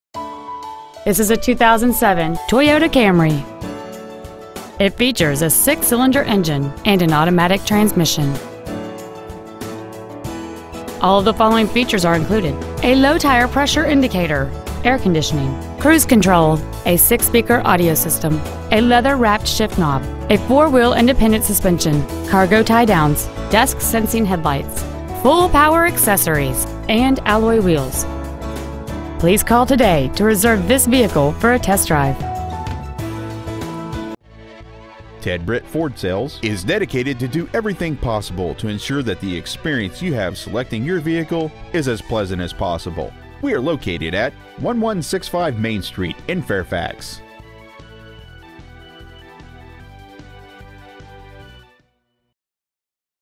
2007 Toyota Camry Fairfax VA
Year : 2007
 Make : Toyota
 Model : Camry
 Engine : 3.5L V6 24V MPFI DOHC
 Trans . : 6-SPEED AUTOMATIC
 Exterior : GRAY
 Miles : 80,887

 2007 Toyota Camry Fairfax  VA